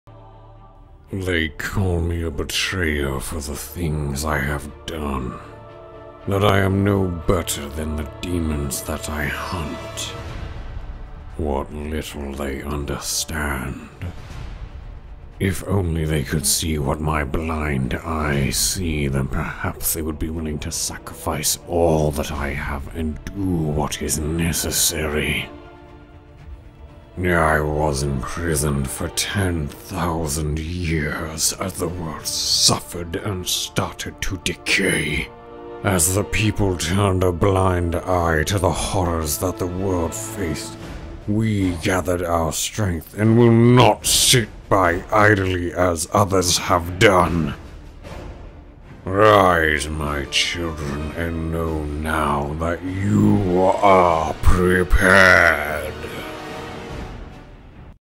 Illidan Stormrage Voice Imitation
A recording I have done in my own home studio of the character Illidan Stormrage, the blind bad boy of the Warcraft world. Originally voiced by Matthew Yang King in 2002 and then by Liam O'Brien in 2004 and since. Love both these actors voices for the character.

I do not own this character nor the rights to the music or art, this is a non for profit demonstration of my range as a voice actor.